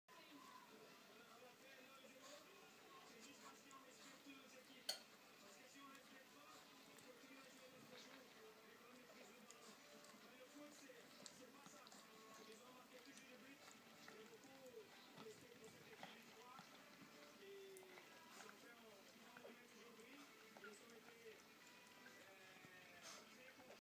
6 chiots sont nés à l'élevage de YORKSHIRE TERRIER à Bastia "Les Toutous de la Rose"
Naissance dimanche 13 mars 2016
Maman : Joyce de la Marquise de Rascagnac
Papa : Icare-Fritz
6 chiots : 4 mâles et 2 femelles